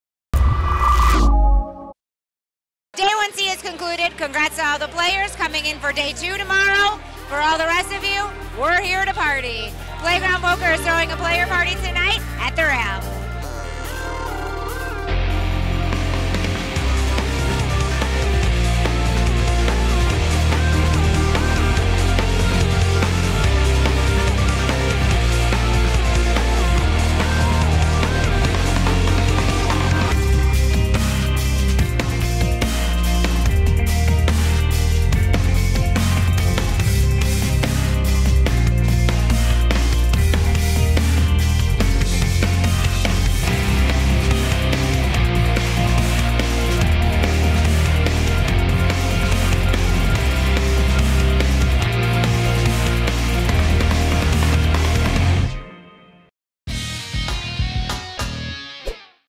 Season XIV partypoker WPT Montreal: Player Party
After three day 1's the players were treated to an open bar with a DJ at The Rail in Playground Poker Club, some celebrated a day two while others were drowning their bust out sorrows.